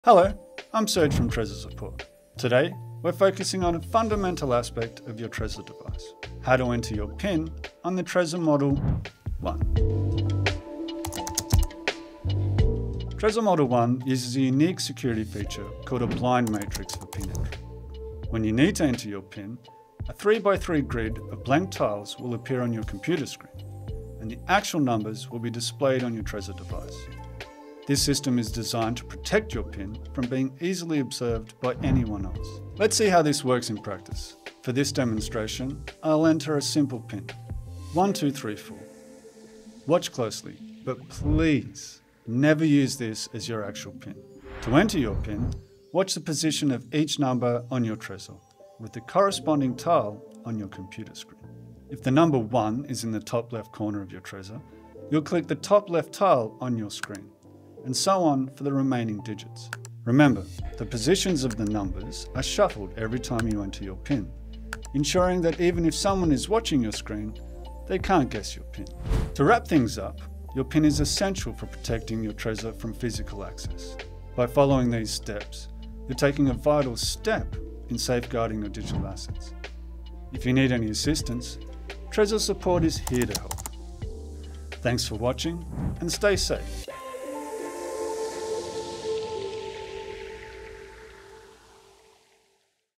Setting Up Your PIN on Trezor Model One | A Step-by-Step Guide
Here's our latest video covering a key security step: how to set up your PIN on the Trezor Model One using its unique blind matrix. This PIN entry method is designed to keep your device secure from anyone watching your screen.

In this video, you’ll learn:
- How the Trezor blind matrix works
- Step-by-step instructions for entering your PIN
- Essential tips to keep your PIN safe